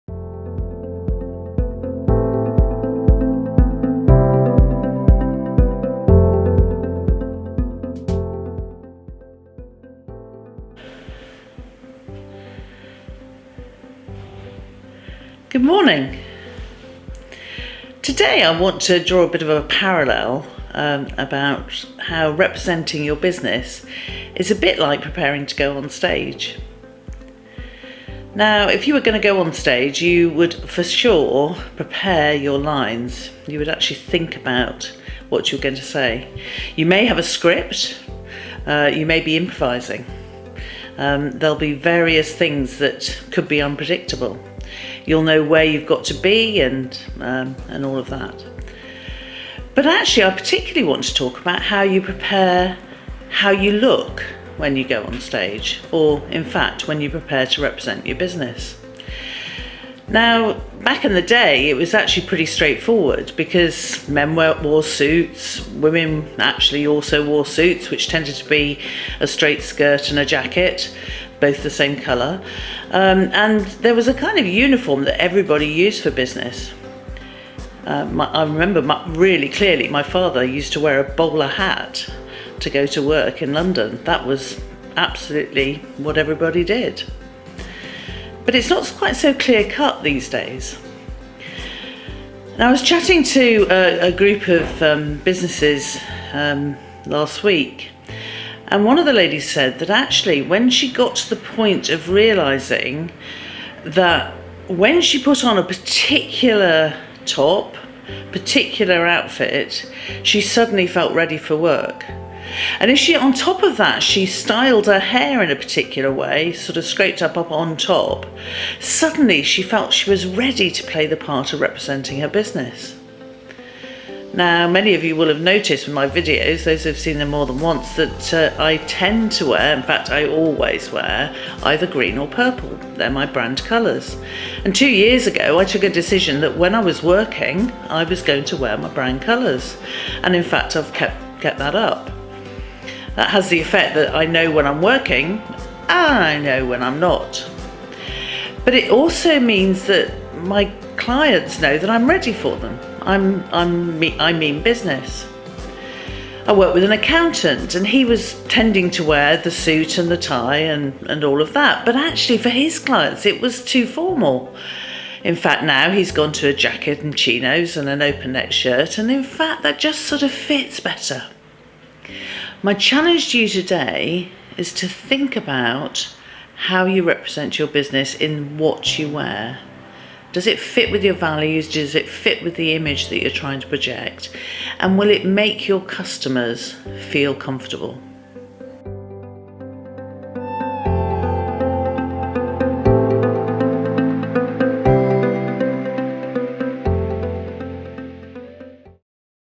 Coffee Break Business Coaching with Debbie Bird (42)
This week I look at being the face of your business and how to feel good inside to represent your business.